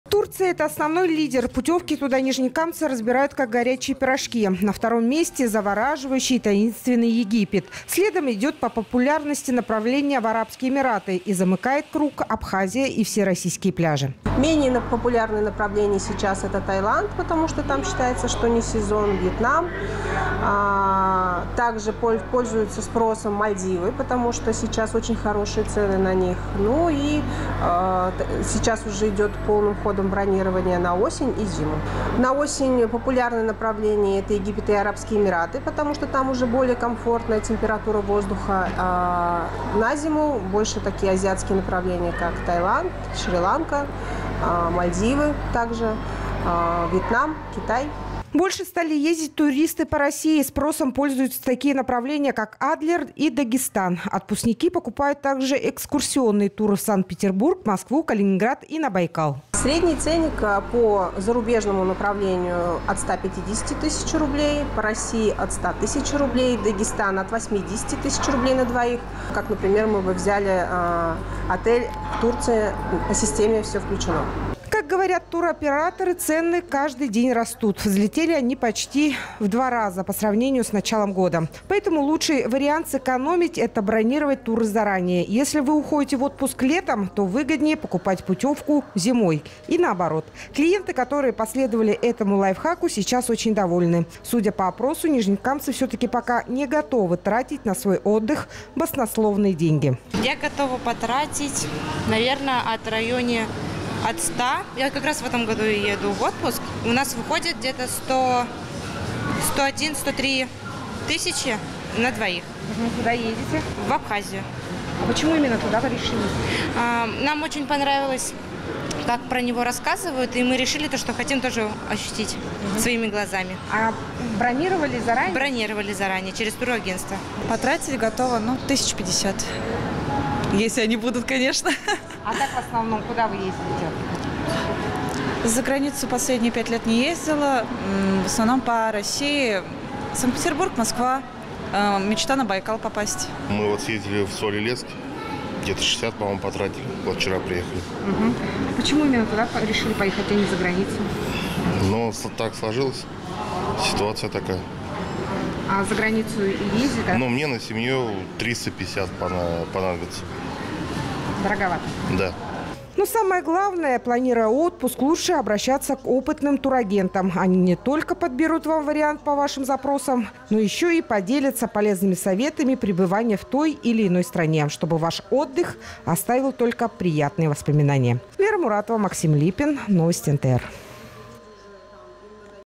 Нижнекамцы чаще всего выбирают отдых в Турции и Египте
Сезон отпусков в самом разгаре. Кто-то из жителей Нижнекамска летит за границу, а кто-то предпочитает путешествовать по родной стране. Цены на туры то растут, то падают.

Турция — это основной лидер. Путевки туда нижнекамцы разбирают как горячие пирожки. На втором месте — завораживающий и таинственный Египет. Следом по популярности идут Арабские Эмираты, а замыкают круг Абхазия и российские пляжи.